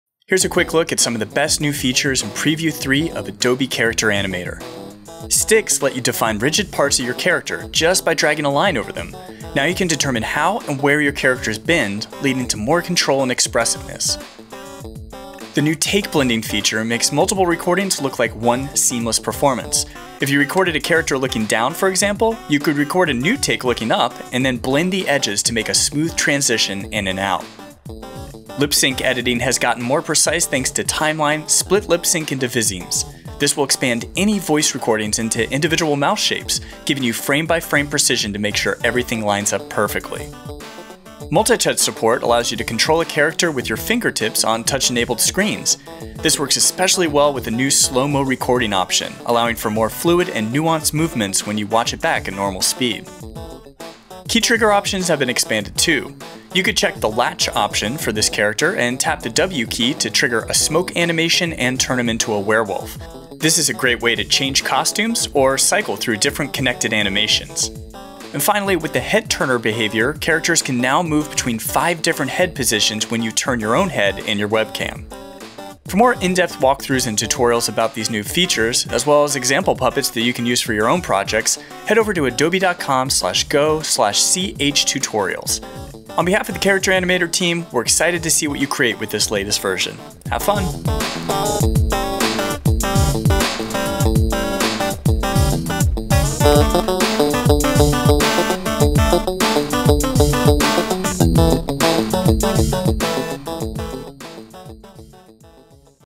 Adobe Character Animator: Preview 3 Overview
Take a quick look at the new features included as part of Adobe Character Animator's Preview 3 release:

• Sticks let you add rigid parts to characters by drawing a line over them.
• Take Blending allows multiple performance capture recordings to blend together seamlessly.
• "Split Lip Sync Into Visemes" gives you a way to edit individual mouth shapes for more precise lip sync tweaking.
• With multi-touch support, you can control a character's draggable parts with your fingers on touch-enabled screens.
• Slow Recording options let you record performances at a slower speed for quicker, more nuanced motions at normal playback speed.
• Latched Key Triggers provide a way for keyboard inputs to do new actions, like make a smoke puff appear and turn a character into a werewolf.
• Head Turner Camera Control allows you to control a character's 5 different head positions with your head in a webcam.

(Official Adobe examples)

I work as a designer at Adobe, but thoughts and opinions are my own.